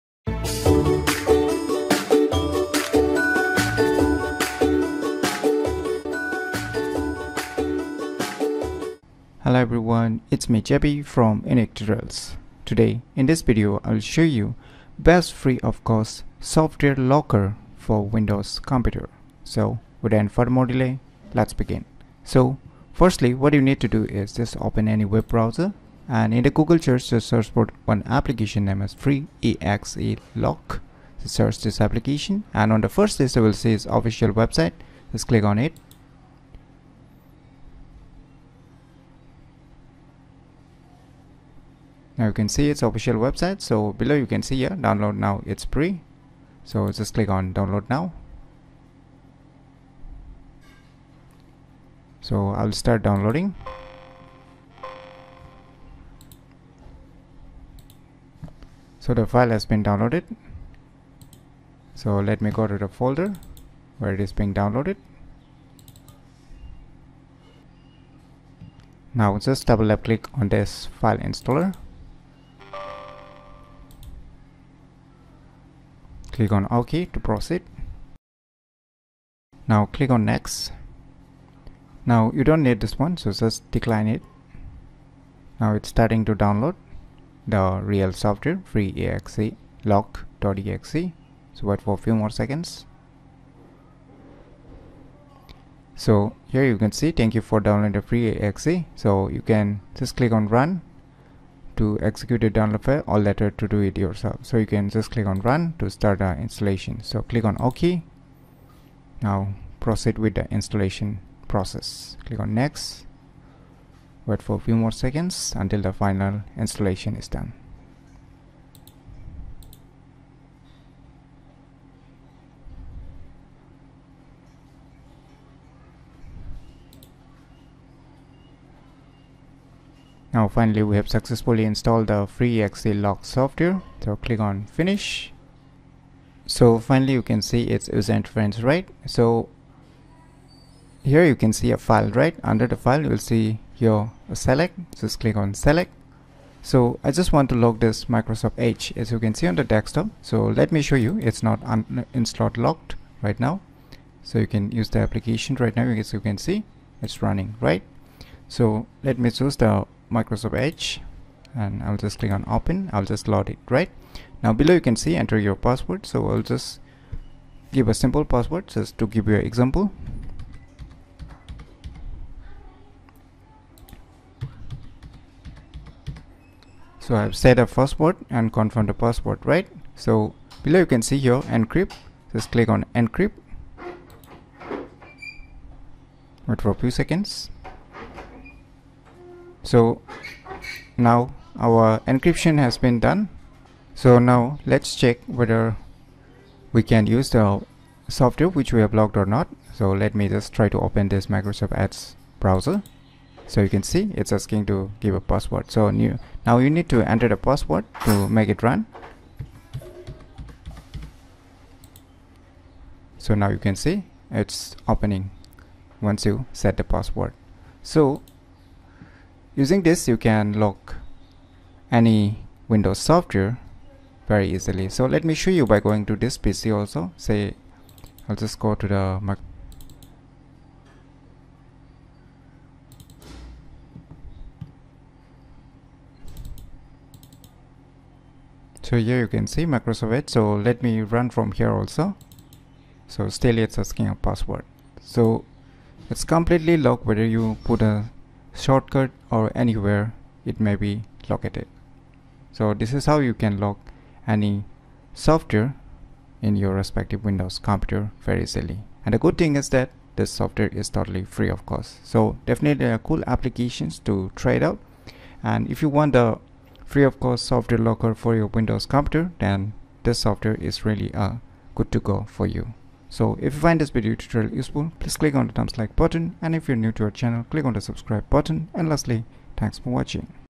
Best Free of Cost " Software Locker " for Windows Computer.
This video tutorial is all about Best Free of Cost " Software Locker " for Windows Computer.